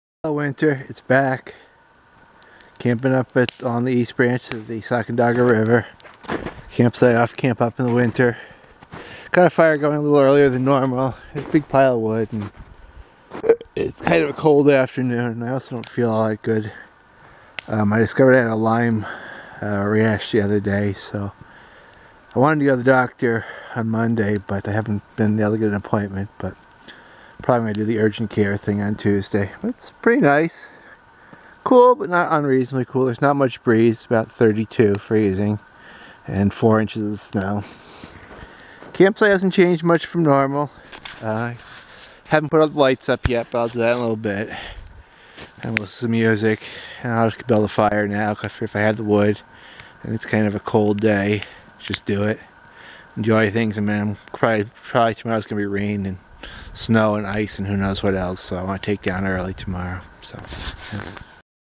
November 23rd Camping at East Branch
In mid-November I went up to the Adirondacks to do some winter camping. It was fairly cool but not terrible for November but maybe a bit more snow then I was hoping for.